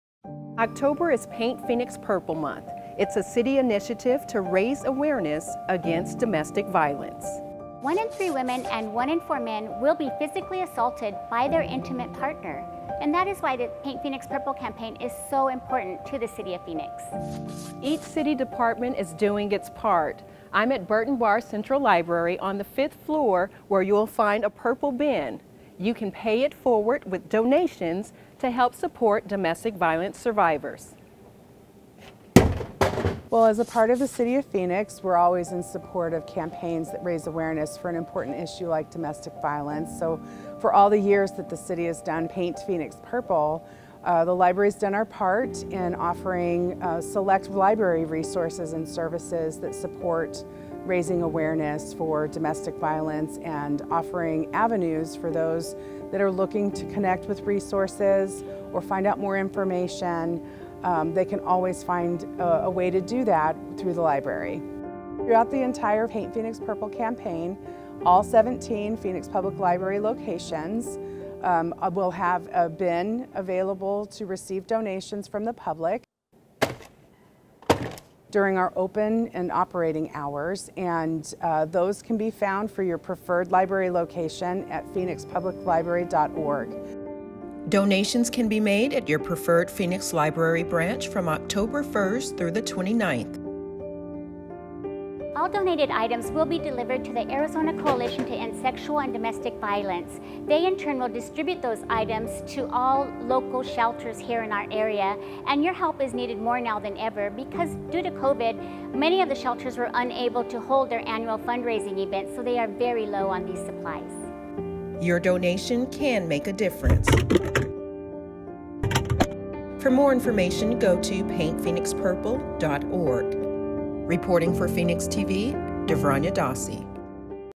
Paint Phoenix Purple Donation Drive at Phoenix Libraries to Benefit Domestic Violence Survivors
October is PaintPhoenixPurple Month. It's a city initiative to raise awareness against DomesticViolence. All city of Phoenix library branches are accepting donations from October 1st - October 29th to help domestic violence survivors. For more information and to see a list of items needed, go to paintphoenixpurple.org

Latest News: Phoenix.gov/Newsroom
Web: Phoenix.gov
Social: @CityofPhoenixAZ on Facebook, Twitter and Instagram